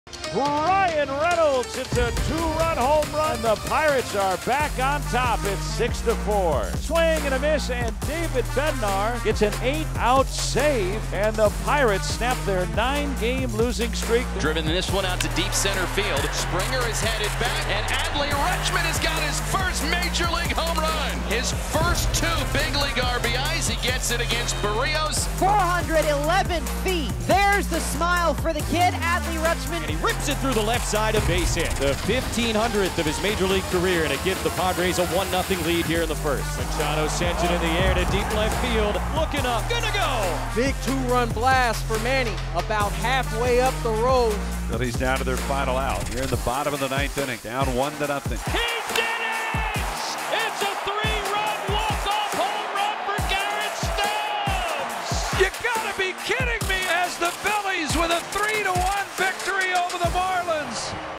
Payoff Pitch presented by DraftKings Sportsbook
See how players and teams around the league fared against the odds on the latest edition of Payoff Pitch presented by DraftKings Sportsbook